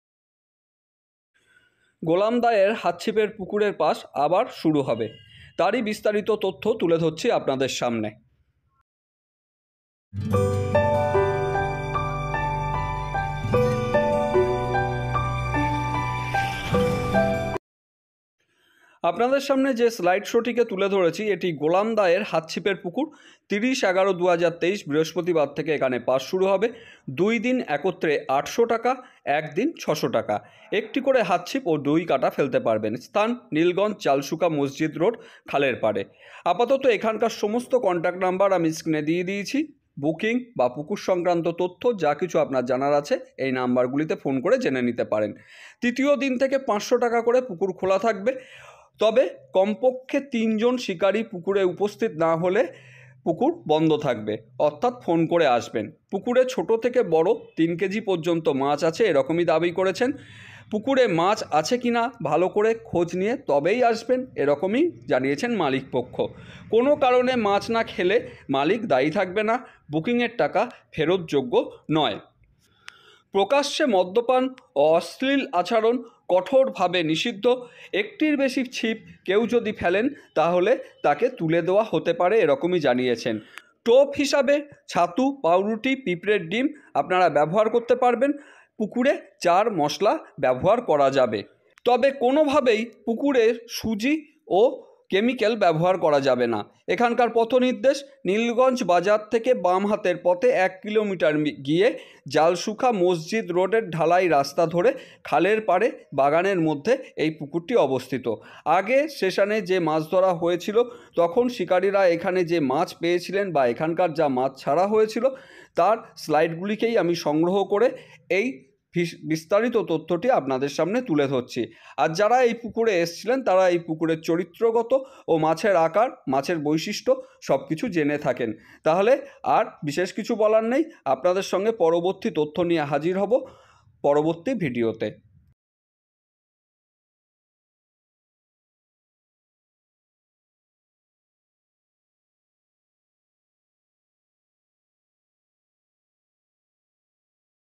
Golam Da Er Pukur Will Be Open Soon- গোলাম দা এর পুকুর আবার খুলছে | BANGLAR MATHE GHATE
Today my channel
@BANGLAR MATHE GHATE
share an information about  Golam Da er  pukur ( jalsukha Nilgange)
Sit prices are 600 rs only
 You can contact these numbers

Thanks for staying with banglar mathe ghate